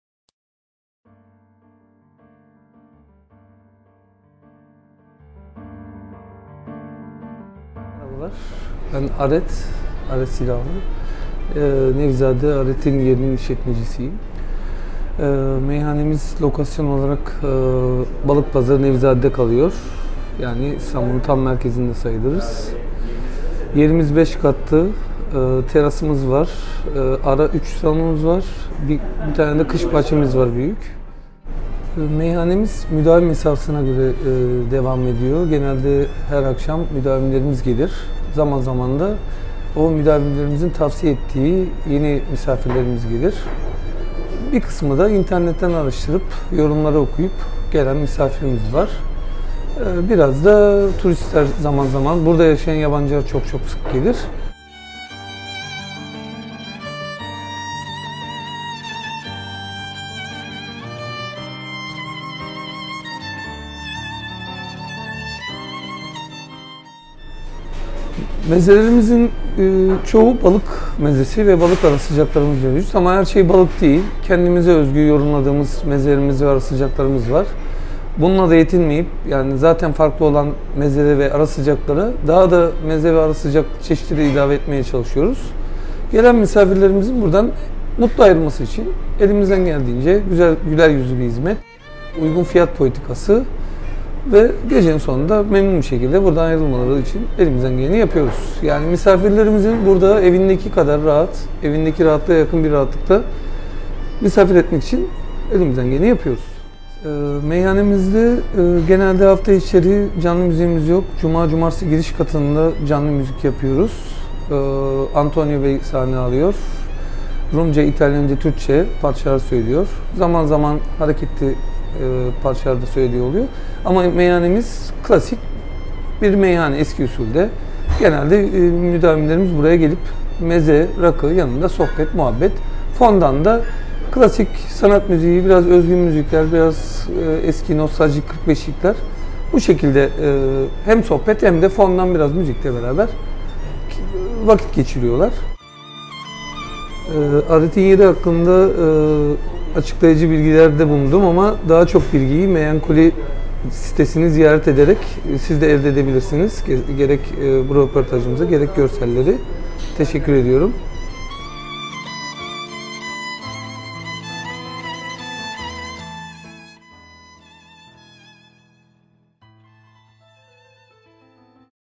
Aret'in Yeri
Aret'in Yeri Beyoğlu'nda,  güzel müzikler eşliğinde rakı sohbetleri ile eski meyhane kültürünü yaşatmayı çalışıyor.

Gelen misafirlerini, evlerindeki kadar kendilerini rahat hissetmelerini ve lezzetli mezelerinden tadıp mekandan öyle ayrılmaları için çalışıyorlar.
5 katlı bir mekan olup en üst katında büyük bir terasa sahip.
Pazar günleri dahil her gün açık olan mekan tüm misafirlerini bekliyor.

🌍Nevizade
⏰ 12:00- 01:00
💸 300₺ (iki kişi ortalama)
🏠 Meyhane
🍽 Balık ve Deniz ürünleri
🥗Meze dolabı
💳 Geçerli